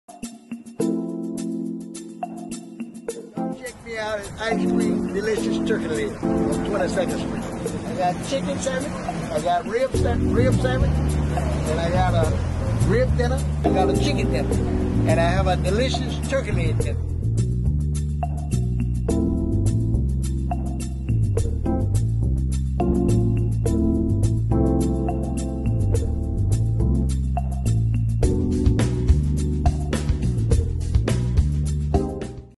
Ice Creams Turkey Legs ! Tampa, Fl
Finest Turkey Legs and BBQ in Tampa, Fl
Located on 22nd ave by Middleton